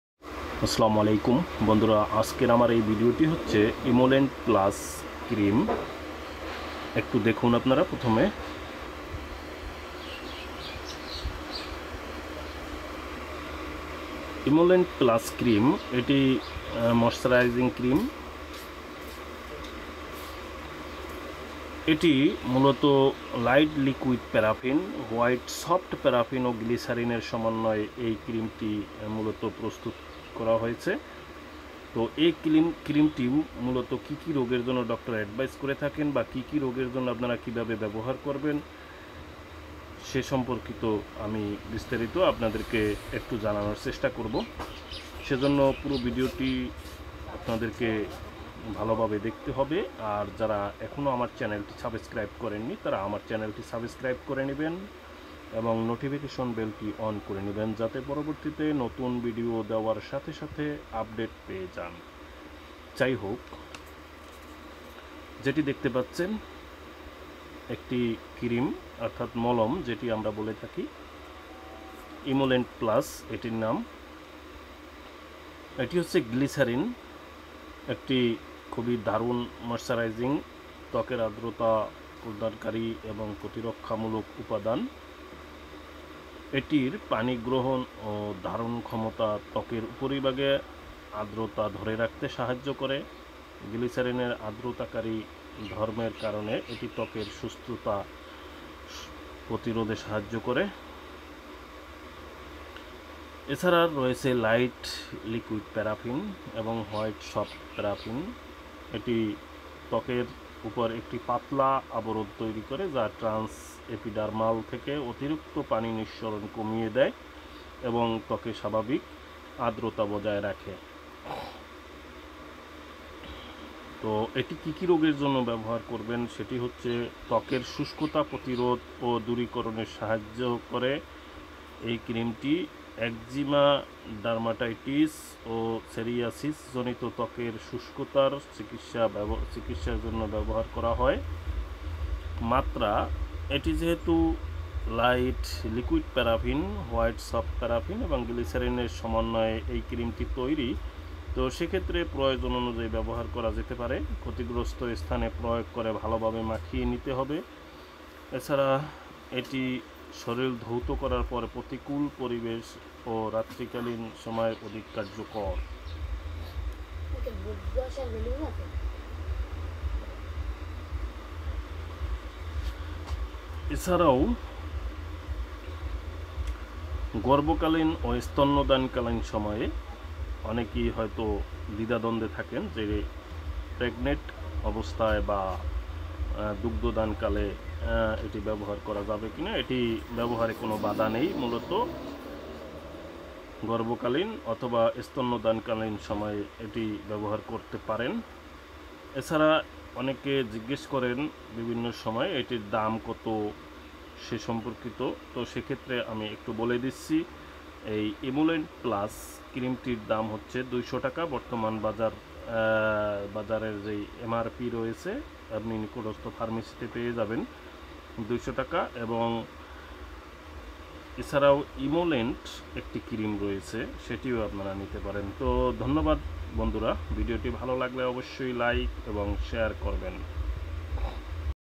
Emolent plus cream । একজিমা ও ময়েশ্চার
Glycerin is an exceptionally good moisturizing, emollient and protective agent. Its ability to capture and retain water slows down the evaporation of water from the surface of the skin. It is also a humectant (wetting agent), which promotes hydration of the skin.

Light Liquid Paraffin and White Soft Paraffin exert an emollient effect by forming an occlusive film which reduces trans-epidermal water loss, thus helping to maintain normal skin humidity levels.

INDICATION

Emolent™ Plus Cream is a highly concentrated moisturizing cream that is indicated

• Prevent and relieve dry skin • Manage dry skin problems associated with eczema, dermatitis and psoriasis